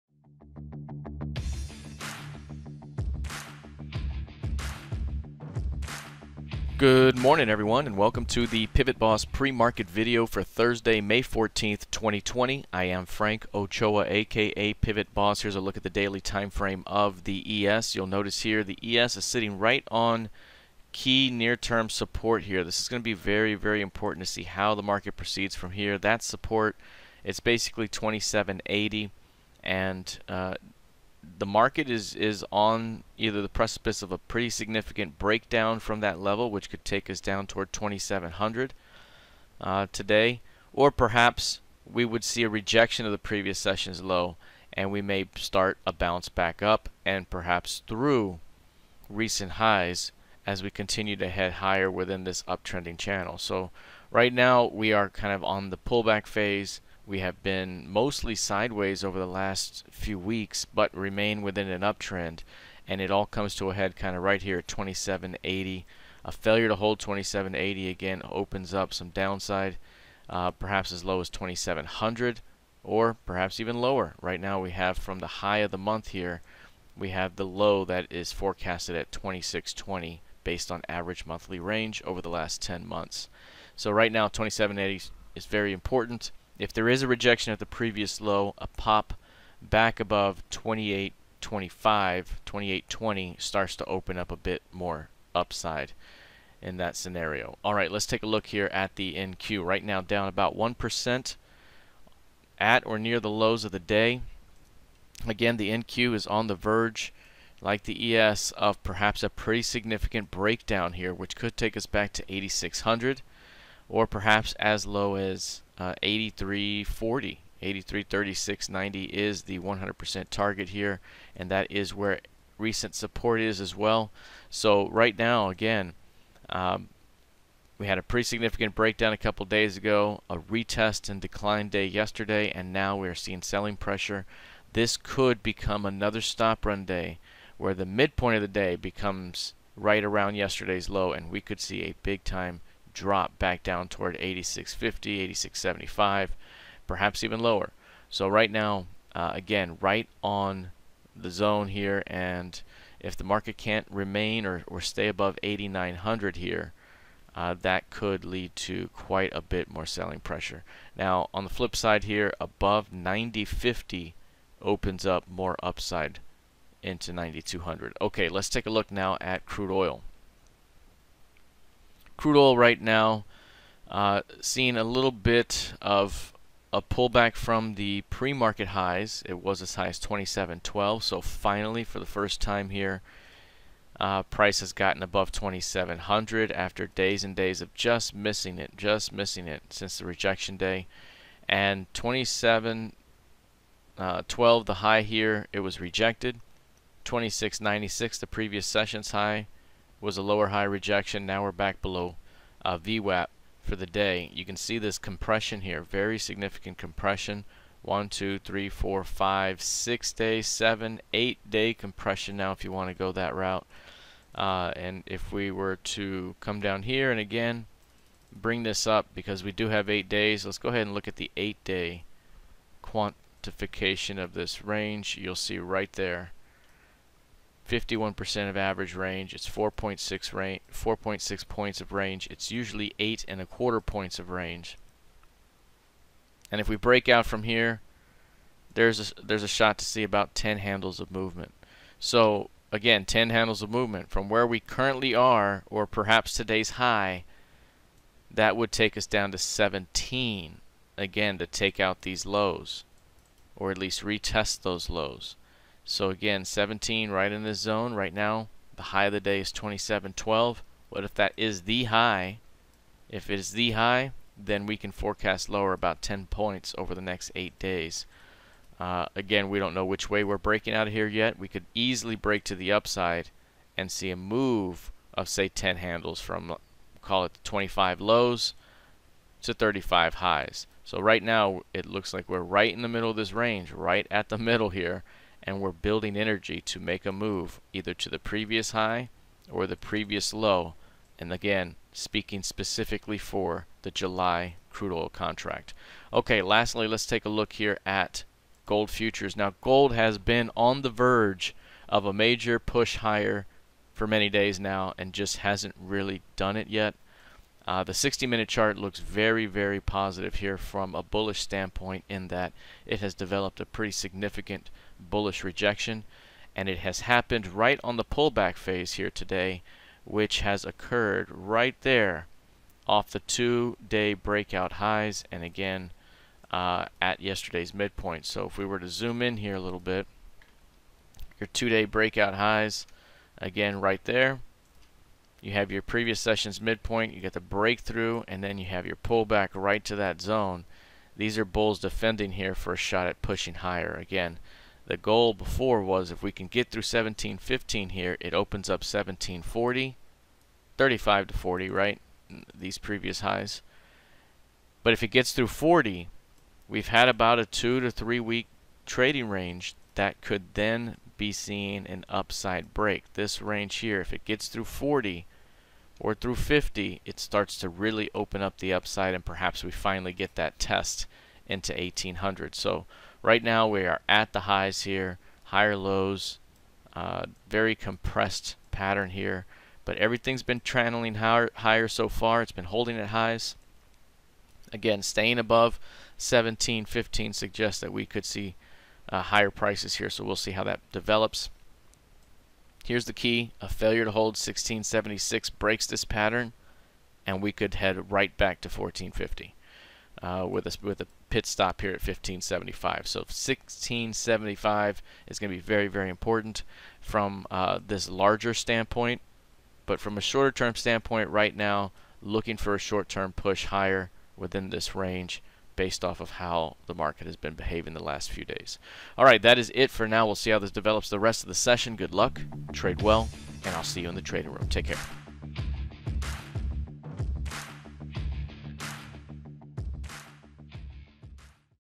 PivotBoss Pre-Market Video [May 14, 2020]: 8-Day Narrow Range for July Crude
May 14, 2020 — THURSDAY AM

NOTE: Thank you very much for joining me for our Monthly Market Outlook webinar, which we hosted FREE in our PivotBoss Echofin Trading Room. To view the recording, log in to our trading room and view the PivotBossTV video playlist.

The ES and NQ are sitting on important short term support levels, which puts these markets on the brink of a big break or rejection. A break down could extend losses into 2700 for the ES and 8500 for the NQ. However, given the overall trend of the market, it's hard not to buy the dips on a pullback of any magnitude. Crude Oil remains within a very narrow 8-day range, which forecasts a breakout of about 10 handles. Gold continues to build toward a squeeze.

Cheers!